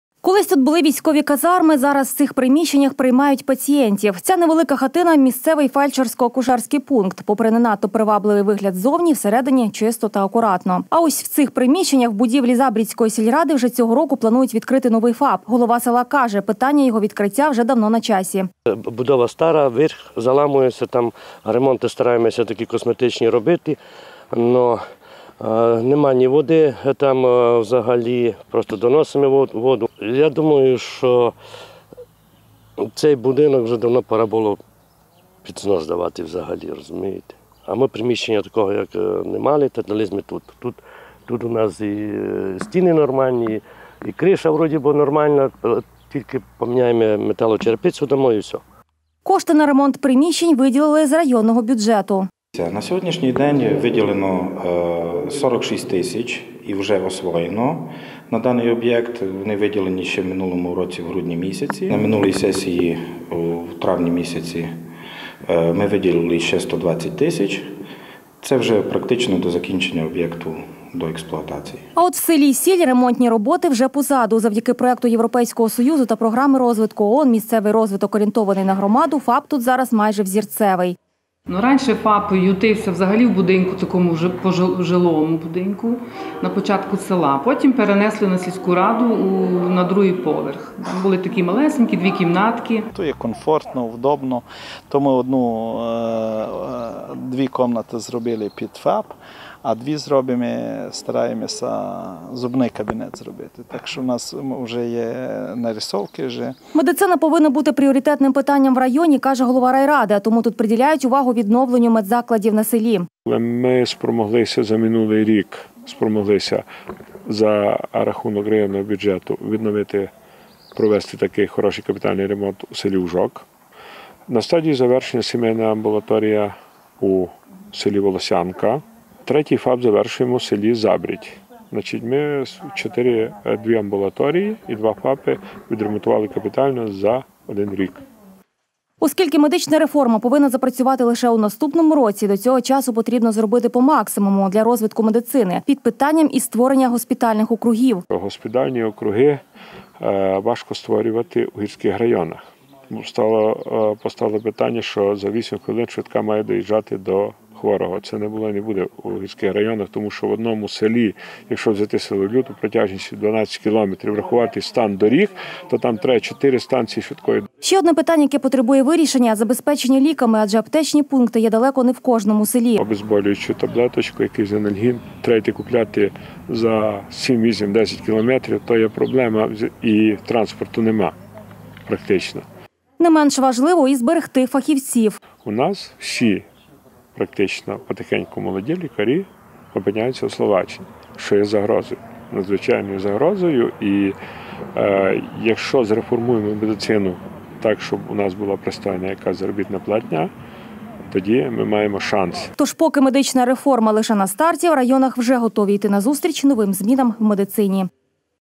Медицина у пріоритеті
Колись тут були військові казарми, зараз, в цих приміщеннях приймають пацієнтів. Ця невелика хатина – місцевий фельдшерсько-акушерський пункт. Попри не надто приваблиий вигляд ззвоні, всередні чисто та акуратно. А ось в цих приміщеннях в будівлі Забрідської сільради вже цього року планують відкрити новий ФАП. Голова села каже, питання його відкриття вже давно на часі. Кошти на ремонт приміщень виділили з районного бюджету. А от в селі Сіль ремонті роботи вже позаду. Завдяки проекту європейського союзу та програми розвитку ОНН «місцевий розвиток орієнтований на громаду» ФАП тут зараз майже взірцевий. Медицина повинна бути пріоритетним питанням в районі, каже голова райради. А тому тут приділяють увагу відновленню медзакладів на селі. Оскільки медична реформа повинна запрацювати лише у наступному році, до цього часу потрібно зробити по максимуму для розвитку медицини. Під питанням і створення госпітальних округів. Ще одне питання, яке потребує вирішення– забезпечення ліками. Адже аптечні пункти є далеко не в кожному селі. Не менш важливо і зберегти і фахівців. Тож, поки медична реформа лише на старті, в районах вже готові іти назустріч новим змінам в медицині.